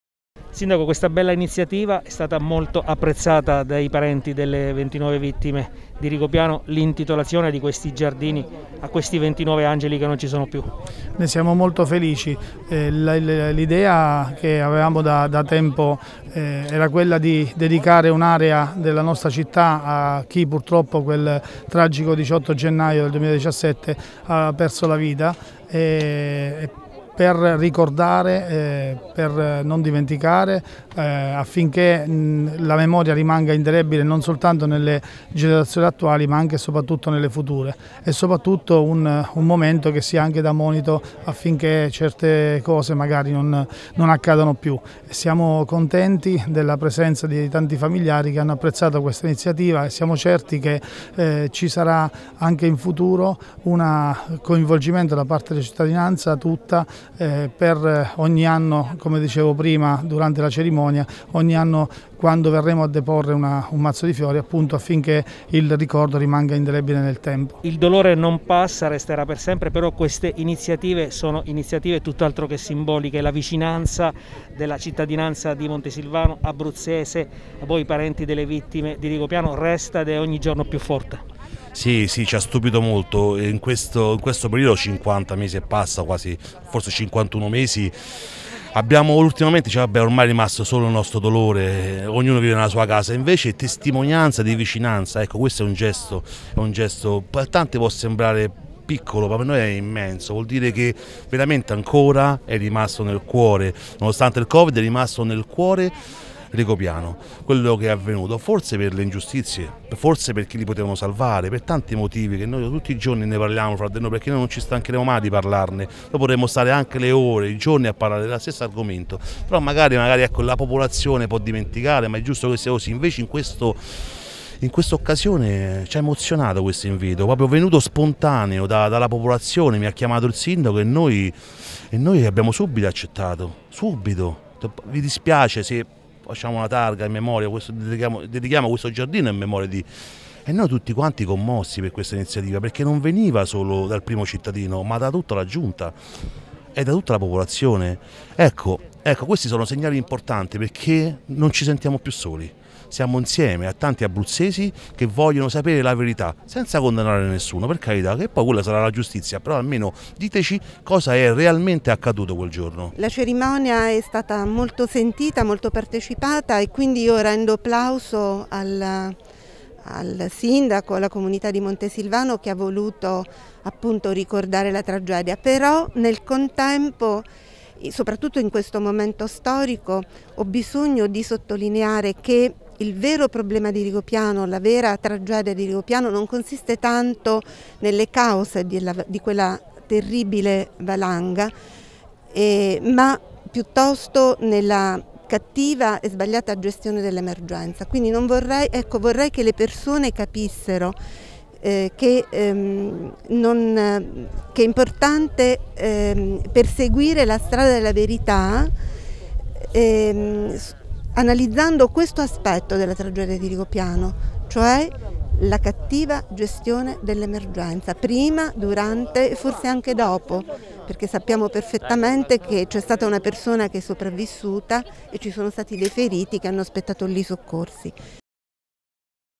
A Montesilvano un giardino in ricordo delle vittime di Rigopiano
C’è stata questa mattina a Montesilvano in via Nilo l’intitolazione del giardino Rigopiano, in ricordo delle vittime della tragedia del 18 gennaio 2017